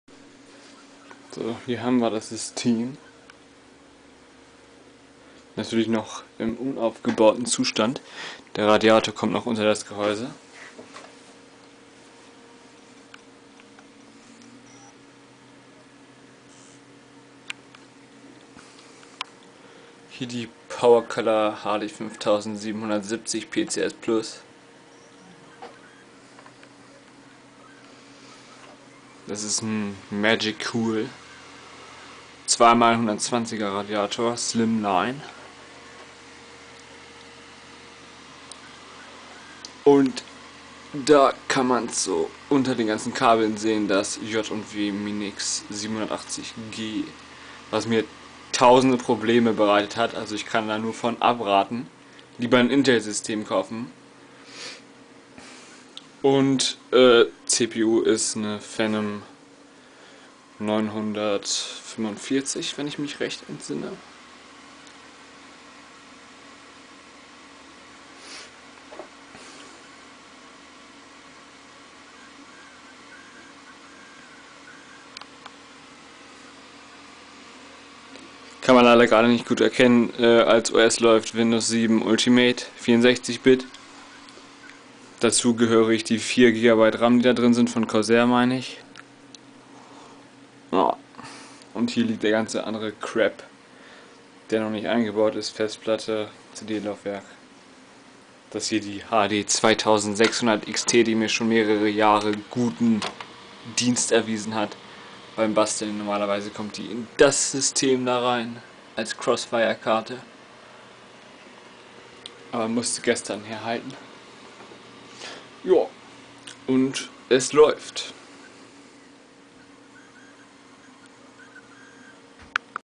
My new watercooled Micro ITX System
Systemstats:
CPU: AMD Athlon II X4 250 (atm 3,0 Ghz standard clock)
Memory: 4 GB DDR2 Corsair
Graphics: Powercolor ATi Radeon HD 5770 PCS+ 1024MB GDDR5
HDD: 500 GB Hitachi 2,5 Inches
DVD RW writer slimline
OS: Windows 7 Ultimate 64Bit

Watercooling:
Pump: Innovatec PCPS
Radiator: 1x 120 mm Magicool single 1x 120mm Magicool dual
Cooled hardware: CPU and Chipsat

The Noise you can hear in the video resuts from my badass mainsystem which is operating in a Antec 1200 case. it has a very mainstream cooling so it is pritty loud. the watercooled system is very very silent. i think it's even more silent than our Home Theatre Shuttle PC with a Q6600 CPU and a passiv cooled grafikscard.

To do: placing the radiator under the case, changing the "fence" on the top of the case with acryl glass.

The other system you can see in the video is my old projekt: mounting a PC in an old metall suitcase.

For the problems i had with the system:

i wanted to mount a scythe 120mm fan (1,5mm depth) on the 120mm radiator inside the case. Put the pump needed to be placed right in front of it so the fan didn't fitt :(

the first mainboard i got was broken
the second did not supported the CPU so i had to take my shitty Phenom I (one) 9600 and place it on the mainboard to make a BIOS-Flash.
After updating to the newest version, the Phenom II X4 945 worked. (although the board officialy supports only 65W CPUs, 95W CPUs are possible to install if you have a good cooling and a little bit look to get the right board which can stand more energy runing thru the board)

After i solved this problem, the installation for windows always crashed with a very nice blue screen of DEATH.

After trying to install Windows for about threebillion times without a graphics card installed by just using the onboard HD 3200 chip, i mounted my nice little HD 2600xt from Sapphire on the board and it worked :)

Excuse my bad english i realy had a though day and now i'm trying to let my mind flow, by listening to my music (which i have uploaded here on youtube)

greetz from Germany,
Vulture_Culture